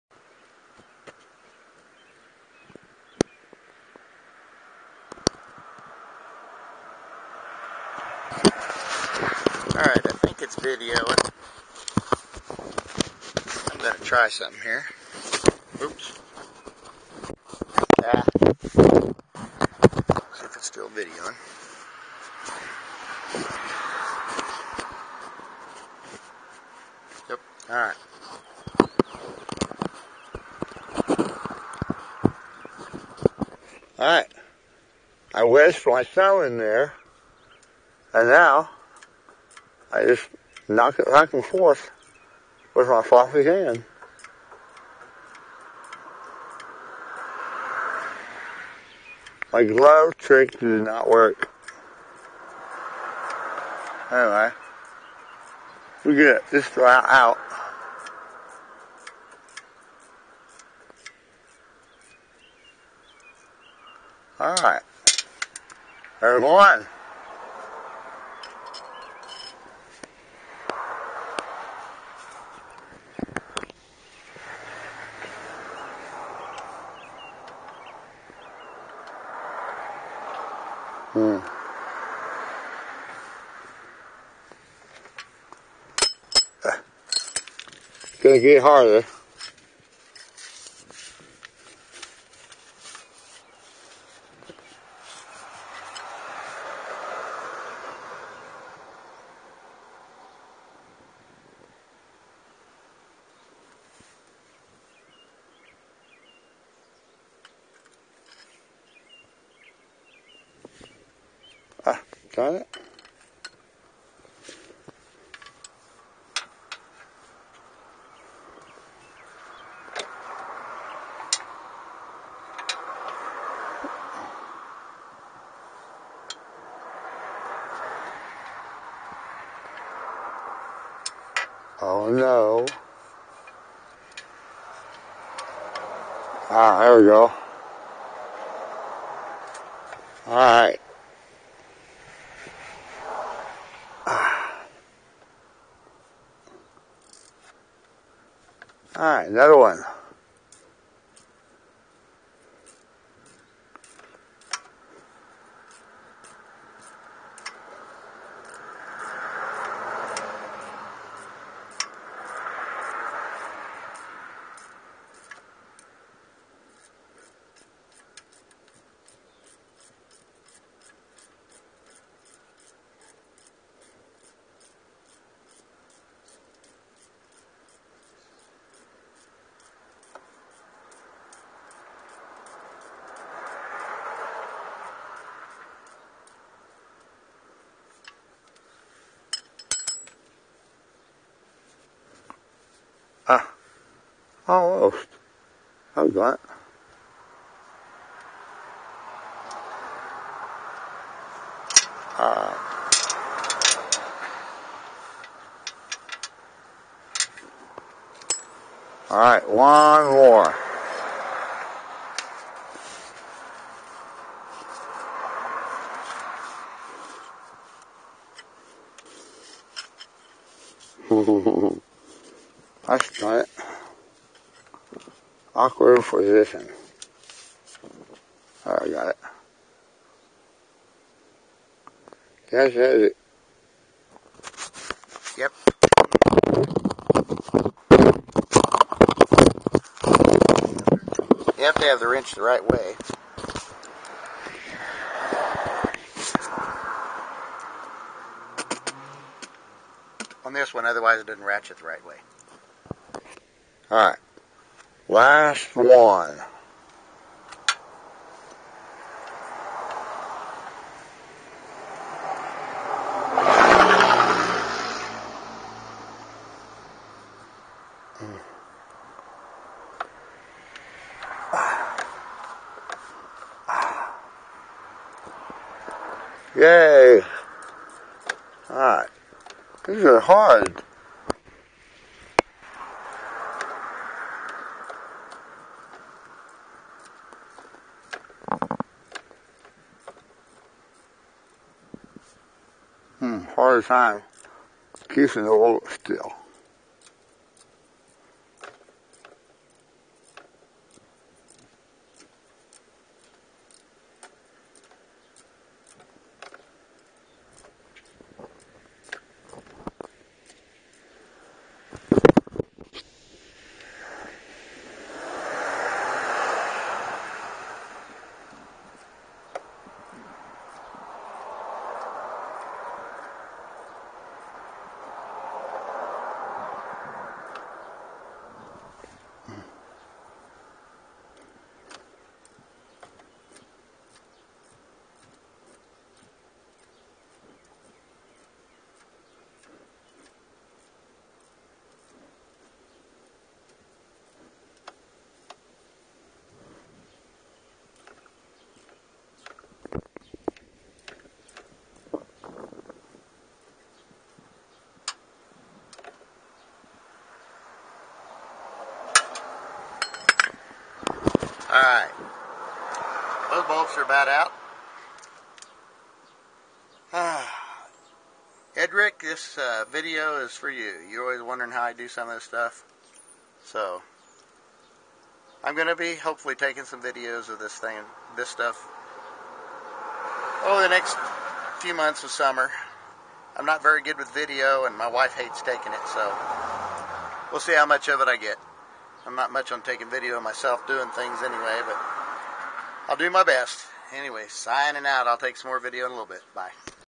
5. C6/7 complete Quad without hand function, pulling the oil pan on a 2004 Saturn Ion
C6/7 complete Quad without hand function, pulling the oil pan on a 2004 Saturn Ion to check Rod Bearings. Was supposed to be a 2.5 hr job for an Able Bodied person but even with my lovely wife getting me tools, turning the motor when needed, and bringing me lunch. it took me 8 hrs.